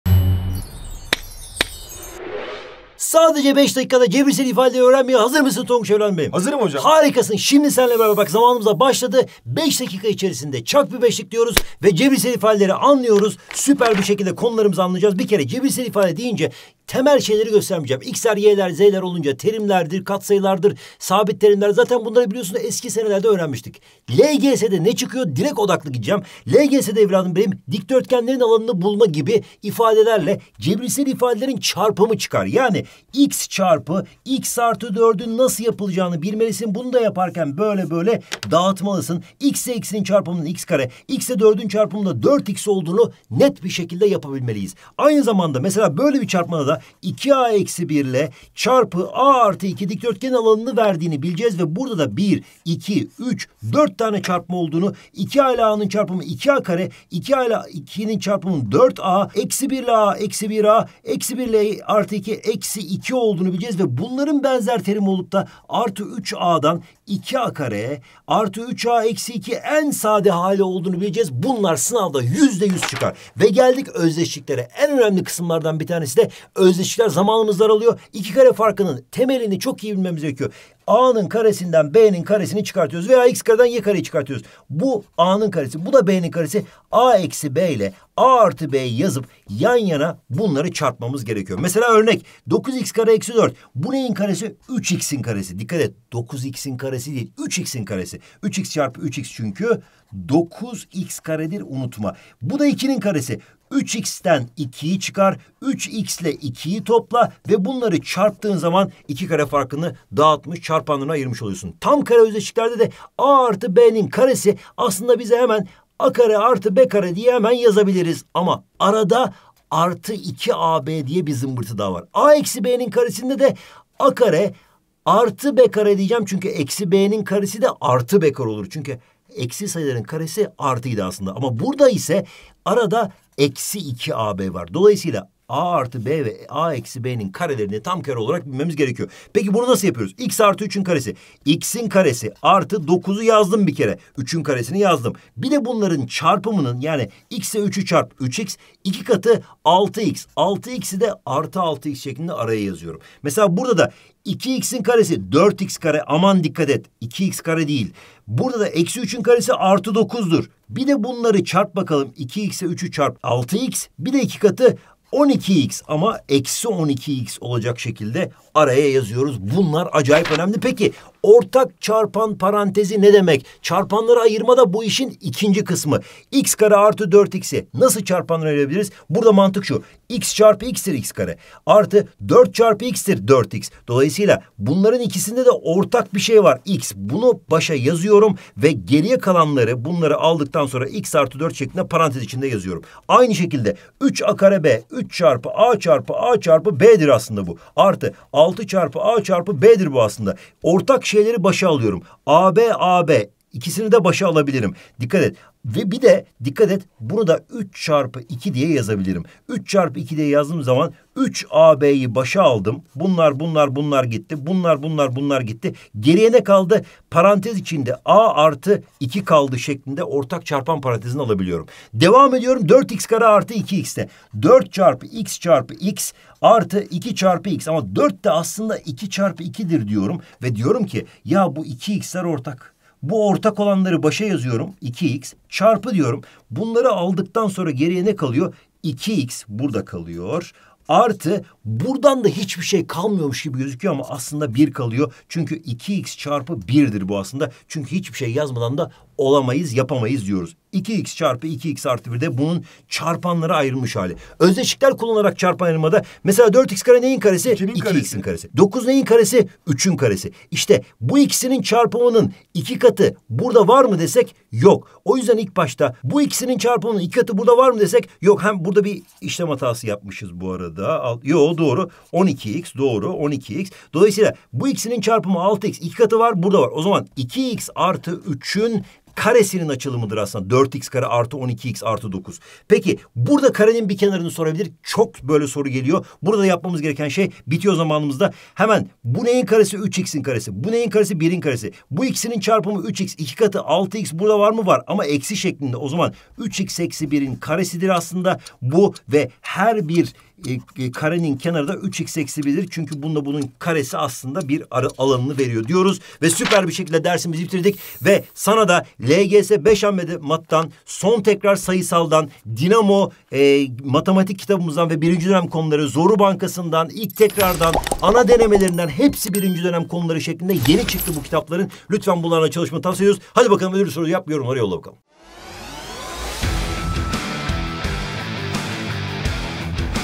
Cebirsel İfadeler 🖐🏻 LGS'de İhtiyacın Olan Her Şey!
📌 5 Hamlede LGS Matematik Konularını Bitir!

"Çak bi 5'lik" serimiz devam ediyor! Bu videoda sadece 5 dakikada 'Cebirsel İfadeler' konusunu öğreniyoruz.
Bir sonraki Çak Bi 5'lik videomuzda hangi konuyu anlatmamızı istersin, yorumlara yazmayı unutma! 👇